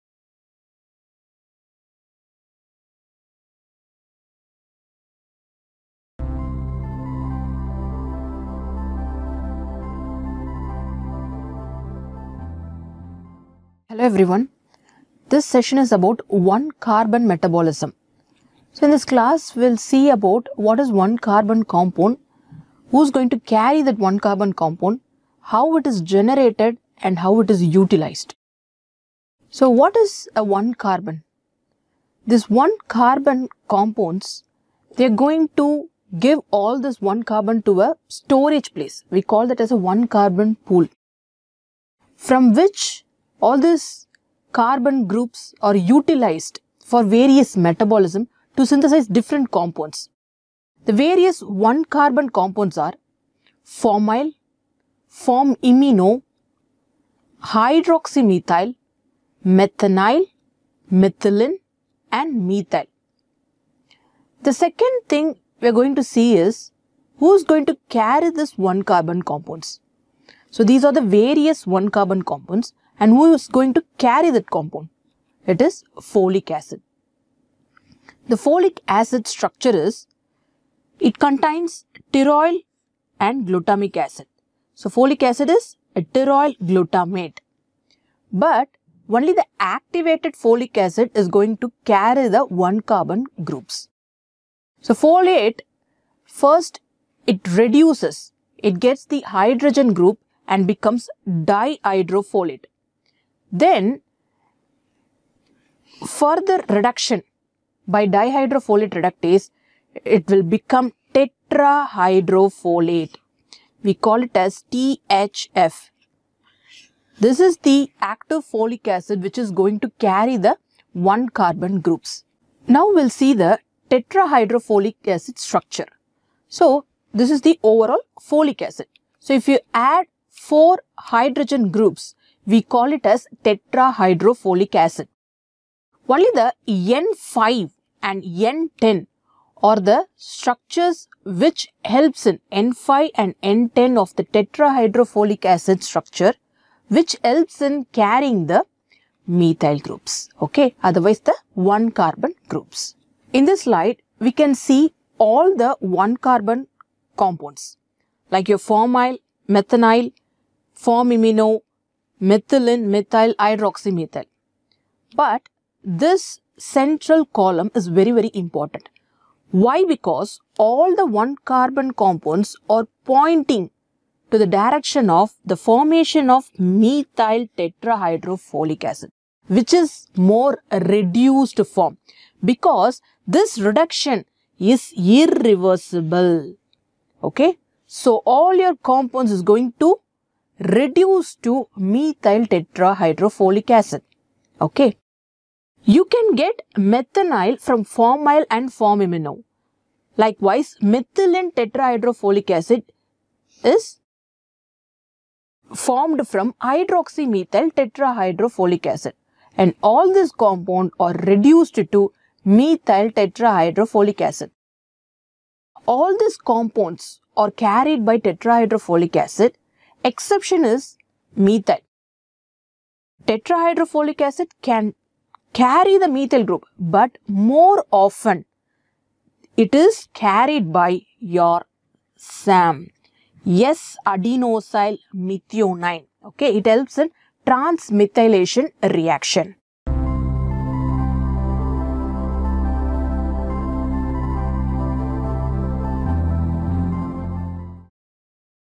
One - Carbon Metabolism
RAAMED App

RAAONLINE App

Description:
The term "protein metabolism" refers to the many biochemical procedures involved in the production of proteins and amino acids as well as the catabolic breakdown of proteins. Transcription, translation, and post-translational modifications are the processes in the production of proteins. Proteins are broken down into smaller peptides, and then finally into amino acids, a process known as protein catabolism. Organic substances known as amino acids have both amino and carboxylic acid functional groups. The process by which proteins are created from amino acids is known as protein anabolism. Based on the NMC CBME curriculum, this module provides comprehensive information about protein metabolism for medical students.